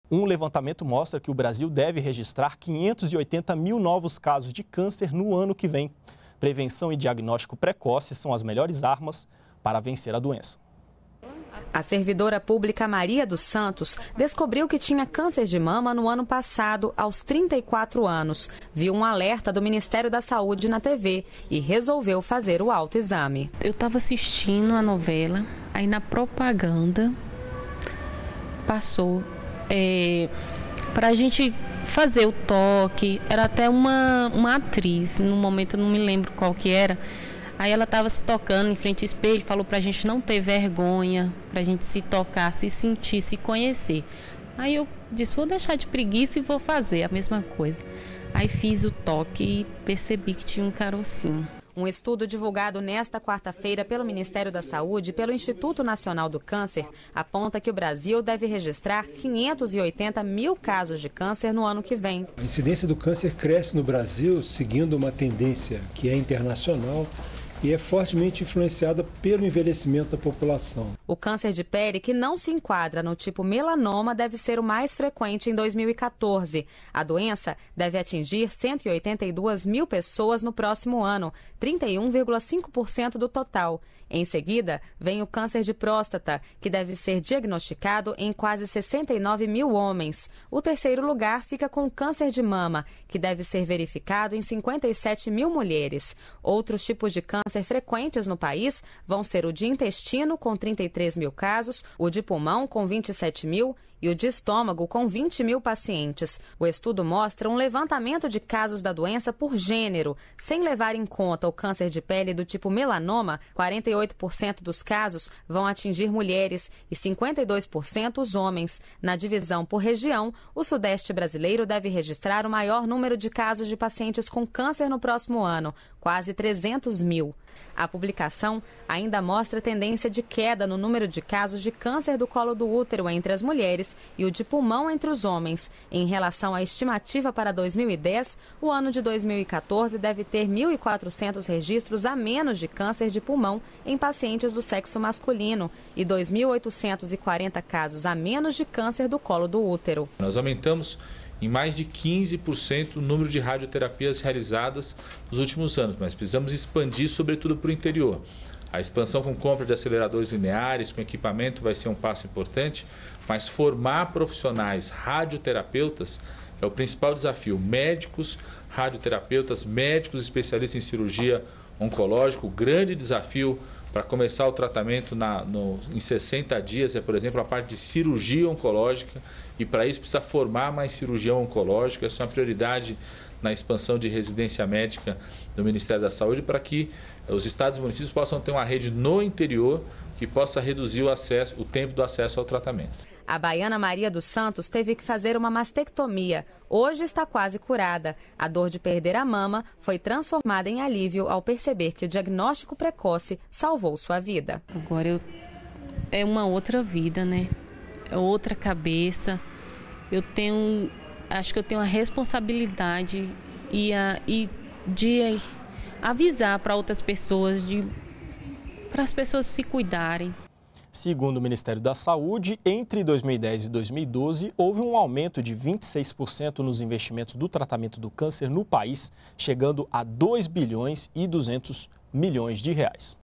Estudo divulga estimativa de incidência de câncer no Brasil em 2014
NBR NOTÍCIAS - 27.11.13: Um levantamento mostra que o Brasil deve registrar 580 mil novos casos de câncer em 2014. A previsão é que o tumor de pele não melanoma atinja 182 mil pessoas no próximo ano, equivalente a 31,5% do total. Câncer de próstata e mama também estão entre os mais frequentes. Os dados são da publicação Estimativa 2014 - Incidência de Câncer no Brasil, que foi apresentada nesta quarta-feira (27) pelo ministro da Saúde, Alexandre Padilha, e pelo coordenador de Prevenção e Vigilância do Instituto Nacional de Câncer José de Alencar Gomas da Silva (INCA), Cláudio Noronha. As previsões de novos casos da doença, divulgadas a cada dois anos, servem de base para políticas públicas na área de oncologia.